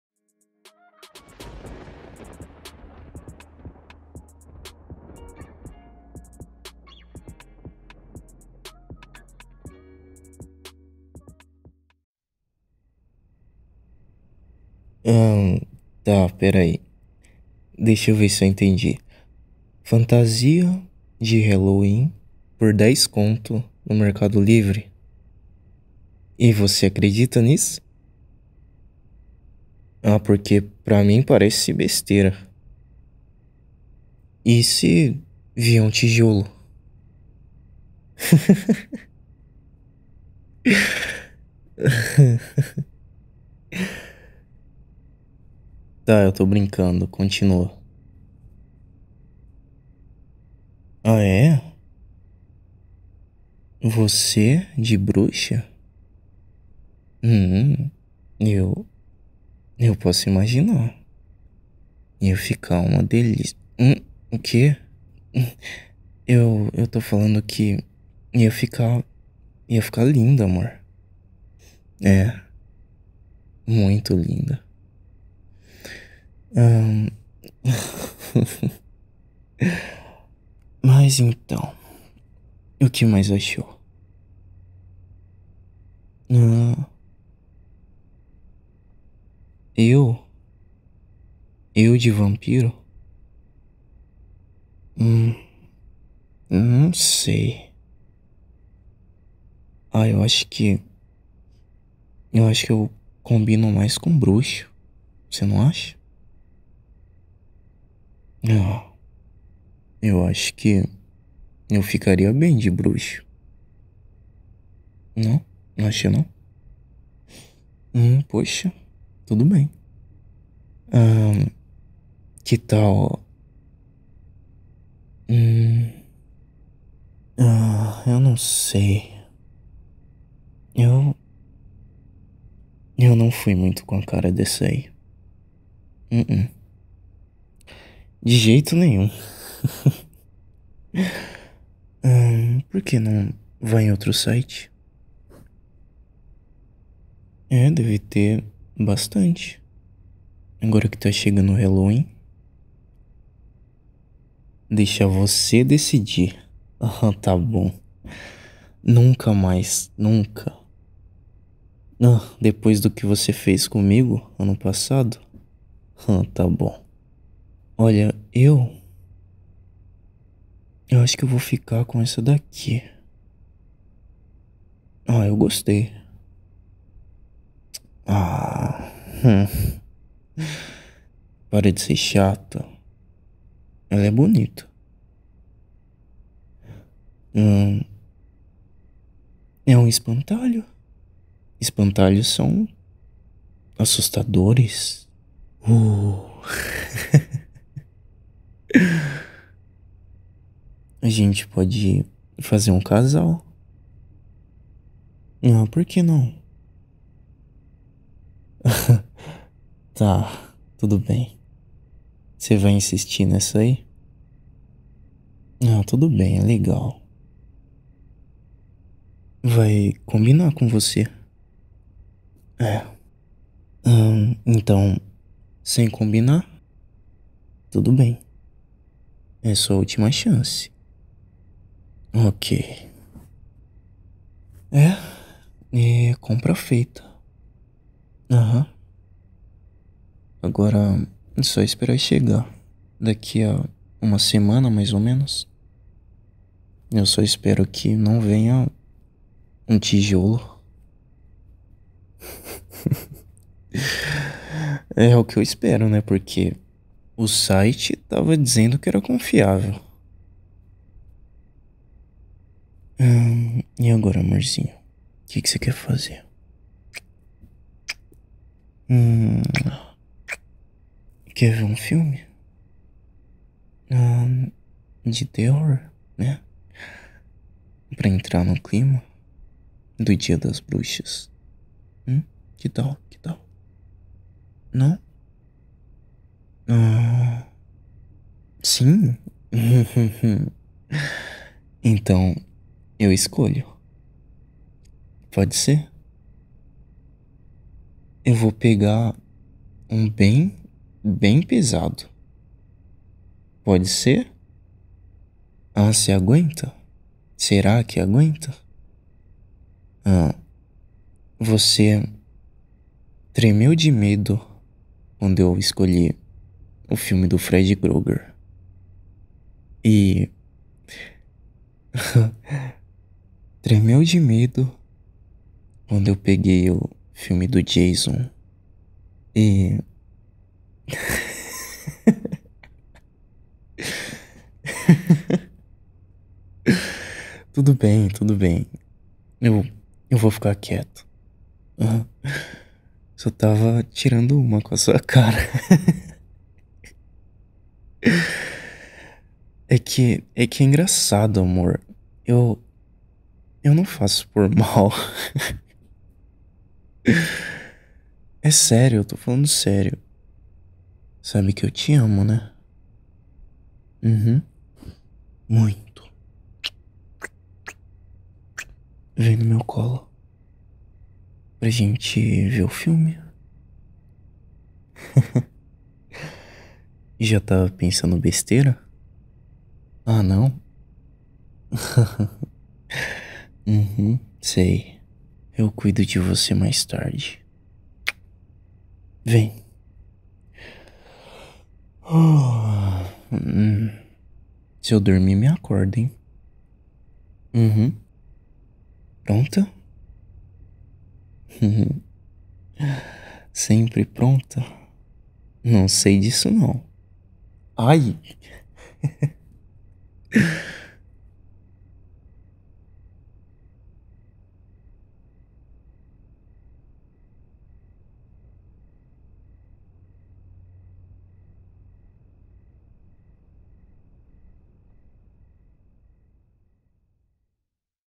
ASMR - SE PREPARANDO PARA O HALLOWEEN COM O NAMORAD0.
Comprando fantasias com o namorado para o Halloween.